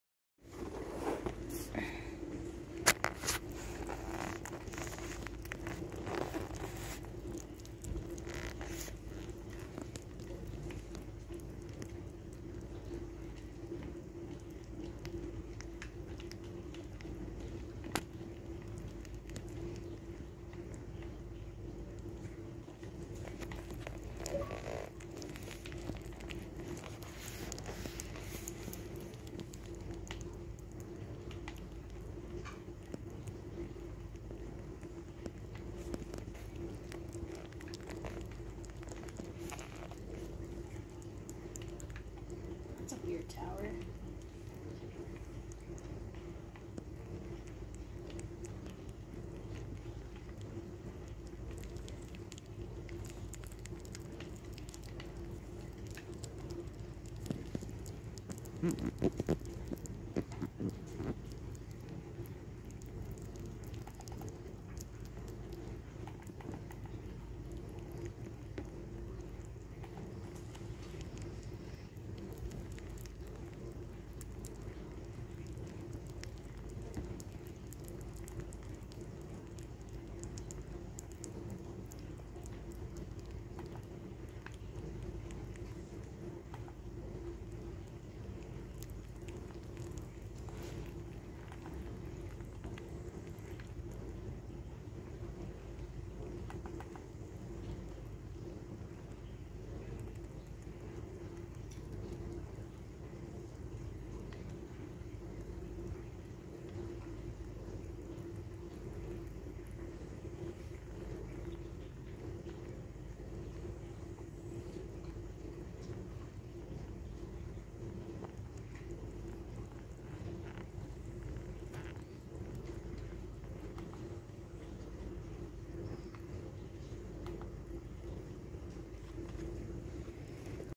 Cute bunny devours banana!!
This is split, She goes crazy for bananas...